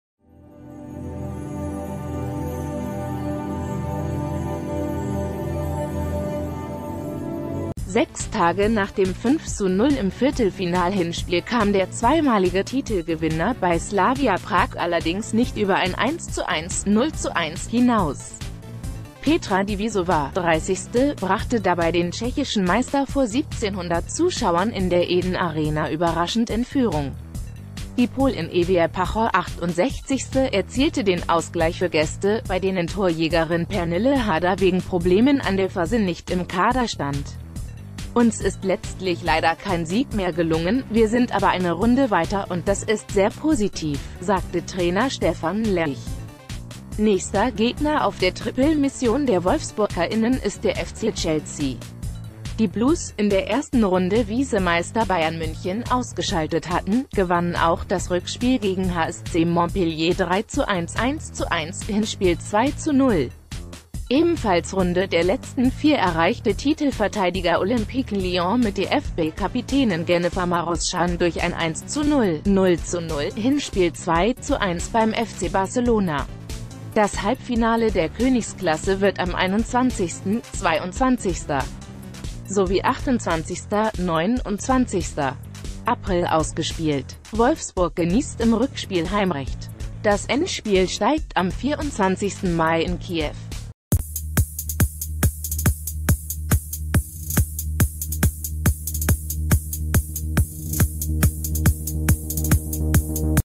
Aktuelle Nachrichten | Champions League: Wolfsburgerinnen im Schongang ins Halbfinale
Sechs Tage nach dem 5:0 im Viertelfinal-Hinspiel kam der zweimalige Titelgewinner bei Slavia Prag allerdings nicht über ein 1:1 (0:1) hinaus. Petra Divisova (30.) brachte dabei den tschechischen Meister vor 1700 Zuschauern in der Eden Arena überraschend in Führung. Die Polin Ewa Pajor (68.) erzielte den Ausgleich für  Gäste, bei denen Torjägerin Pernille Harder wegen Problemen an der Ferse nicht im Kader stand. "Uns ist letztlich leider kein Sieg mehr gelungen, wir sind aber eine Runde weiter un...